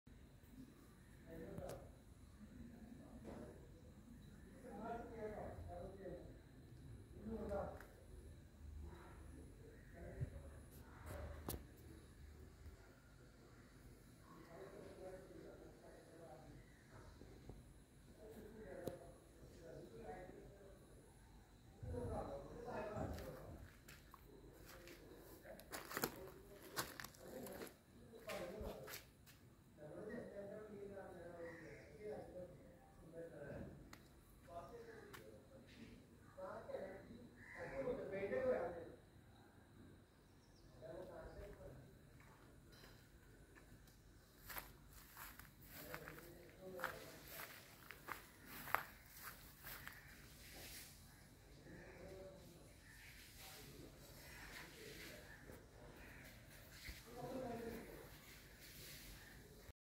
6 cents 1900 sqft at vadakke kidangoor angamaly price 6500000 /.
6 cents land with 1900 sqft house at kidangoor angamaly area,
200 mtr f rtarring rd,500 mtr to mookkannoor bus route.
pakka karaboomi,flood not effected,plenty and pure well water.
4 km rail way,9 km airport,
300 mtr rc church,600 mtr jacibite church also temple.
1.5 km schools,3 km mookkannoor hospital fecilitty.
3 km Angamaly city,2high level pottential area.20 km metro station aluva,
reputed famalies staying barea.
very strong and perfect construction, used with branded fittings
Used with durable woods.